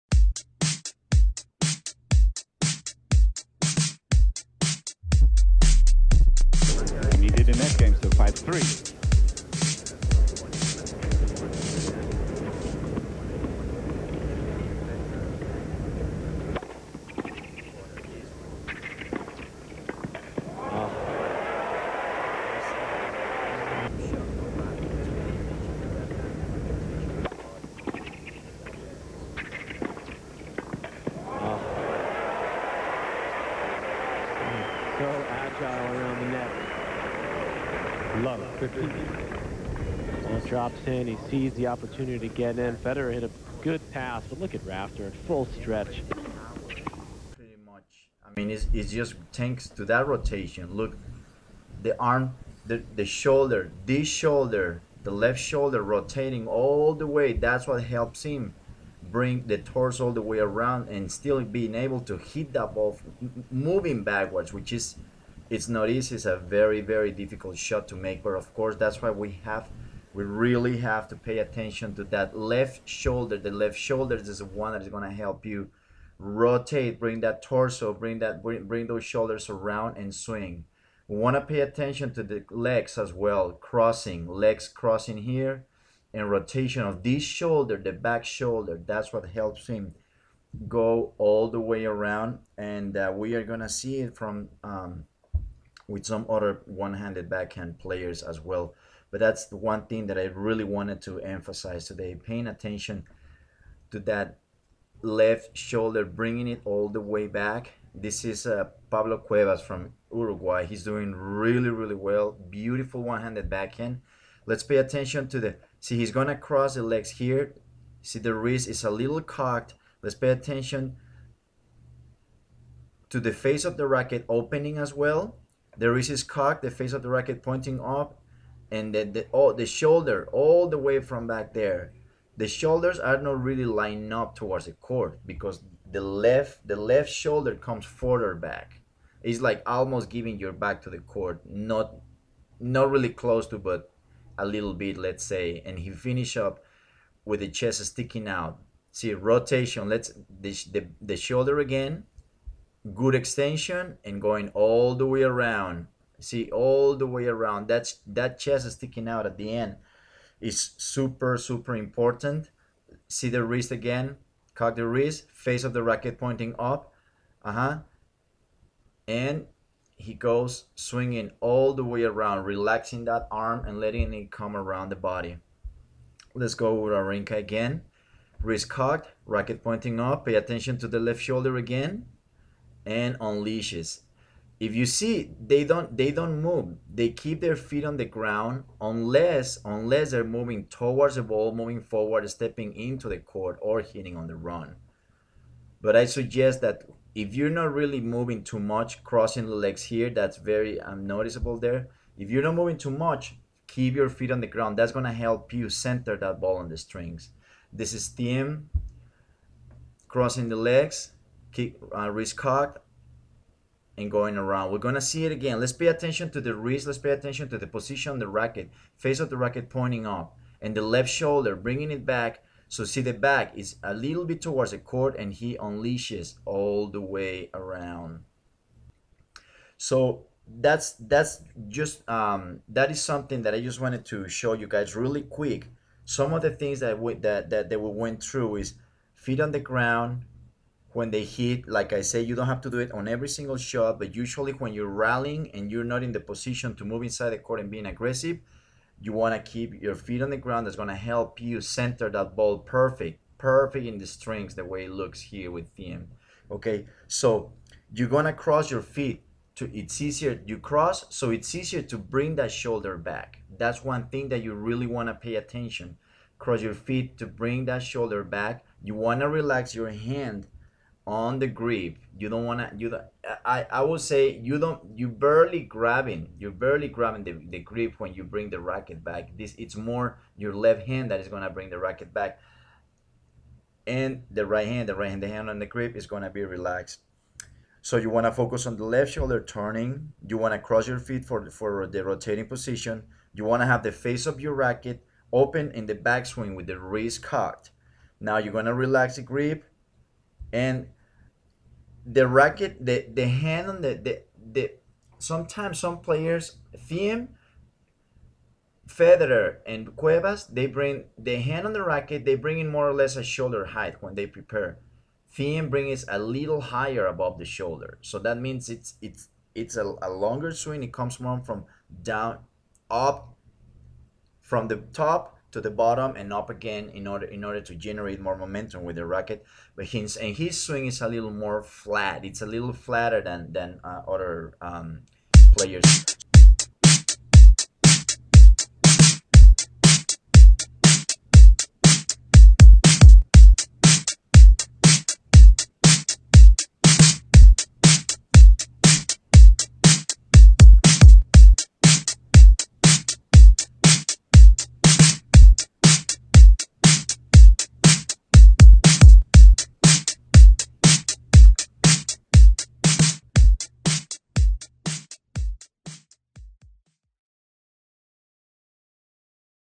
One Handed BH: How ATP Bombers Detonate Their Charges
Check my other YouTube channel Arise by Mauro Gallardo